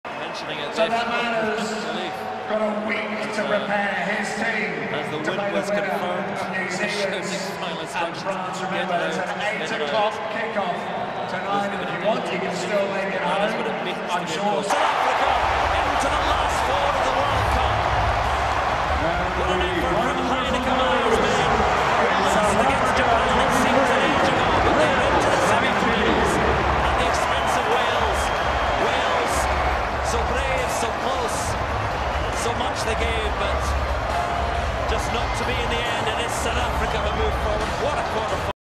South Africa Coach's wild winning reaction!
This is what it means to win: Heyneke Meyer's reaction to final whistle as the Springboks  go through into the semi-finals after defeating the Welsh after a very competitive match.